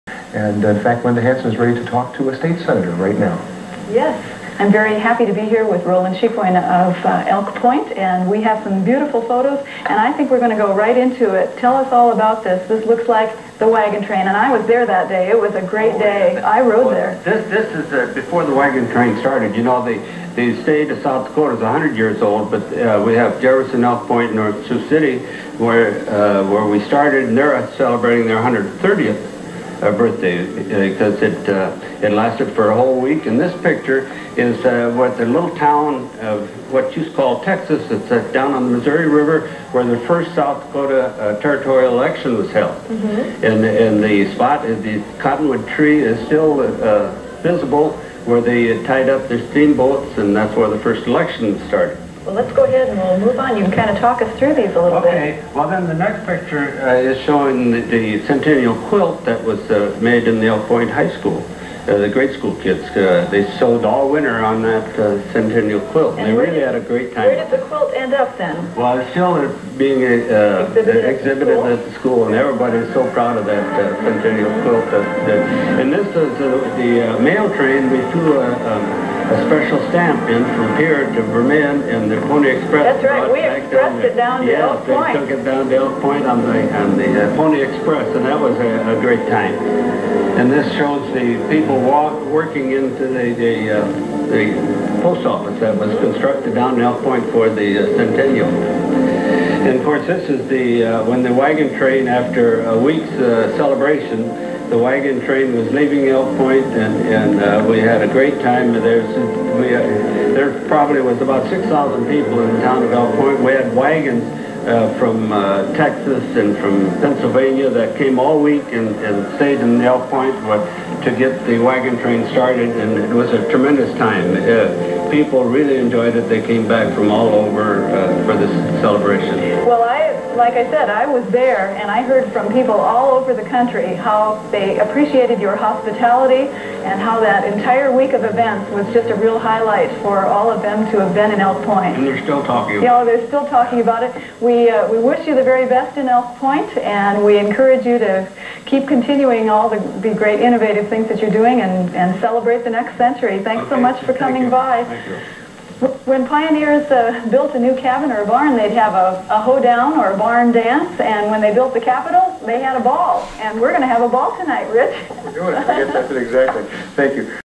CMGUS VCR CLASSIC: 1989 NOV 4 SD PBS SDPTV ROLAND CHICOINE ELK POINT HISTORY CENTENNIAL FINALE CLIP
CMGUS VCR CLASSIC: 1989 NOVEMBER 4 SD PBS SDPTV ROLAND CHICOINE ELK POINT HISTORY BEFORE THE DANCE BALL SOUTH DAKOTA 100 BIRTHDAY SHOWN DURING THE PIERRE CAPITOL CENTENNIAL FINALE

NOTES: 1989 NOVEMBER 4 SD PBS CENTENNIAL CELEBRATION 100 YEARS BIRTHDAY STATEHOOD PIERRE PARADE & CAPITOL BALL FINALE ALL CLIPS & COMMERCIALS SDPTV SOUTH DAKOTA PUBLIC TV HOST CREW CREW RICHARD MULLER LINDA HANSEN GENNA LOCKHAART LARRY ROHRER GINGER THOMSON TODD EPP ETC * (SOME BAD AUDIO PARTS DUE TO OLD VHS TAPE & BAD PBS AUDIO/MICROPHONES DURING THE BALL)